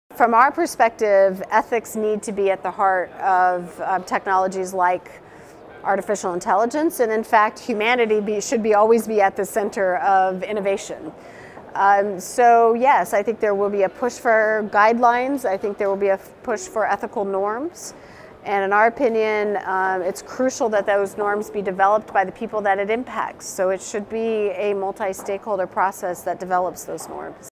Global IGF 2017: Sally Wentworth on Standards for Security and Ethics
Sally Wentworth, vice president of global policy development for Internet Society, shares their answer to the question, “In the next 10 years, will there be commonly accepted global guidelines for security and ethics for the Internet of Things, big data and artificial intelligence?” Recorded at the Global Internet Governance Forum 2017 in Geneva, Switzerland, by the Imagining the Internet Center, an initiative of Elon University, North Carolina, USA.